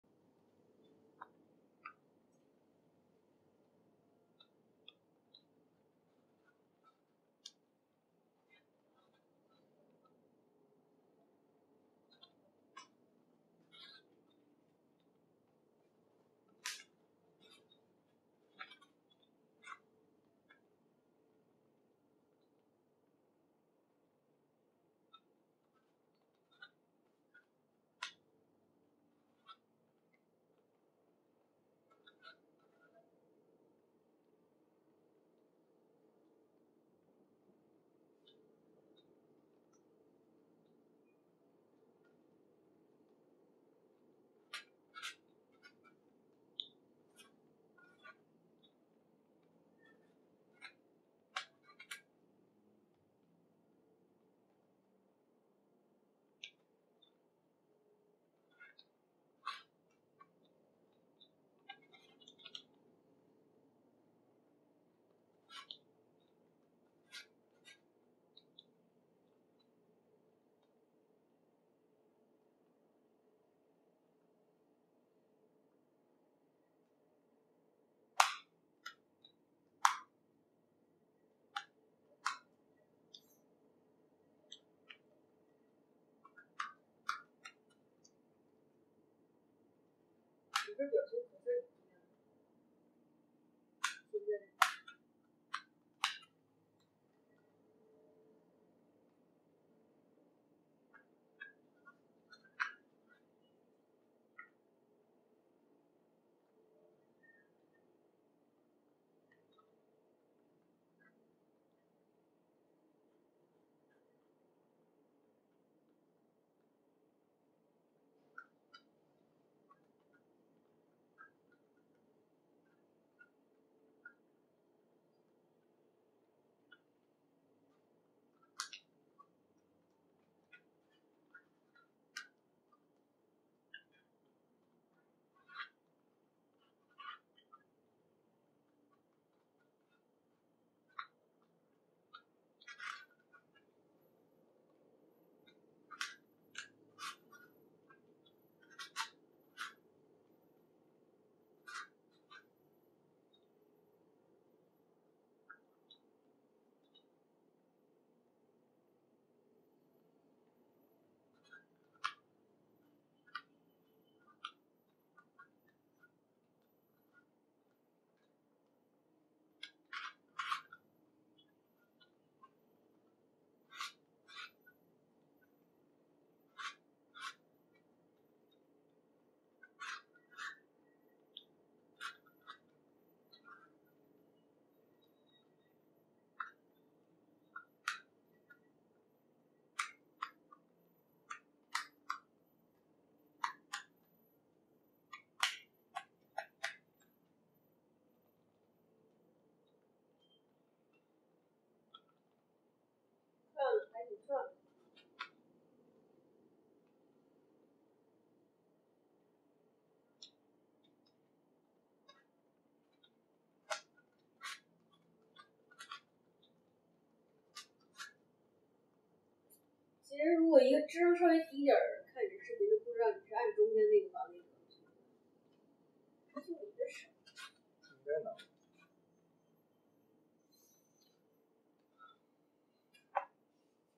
combination locked cabinet
This is the box for my proposal ring in the day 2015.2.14.
It is a Rubik's Cube style puzzle cabinet designed by me and fabricated via Shapeways. The cabinet can only be opened with a sequence of center parts shifting after the cube is fully solved. A drawer with ~ 27 x 27 x 45 mm inner space will be pull out for around 40 mm when it is fully opened.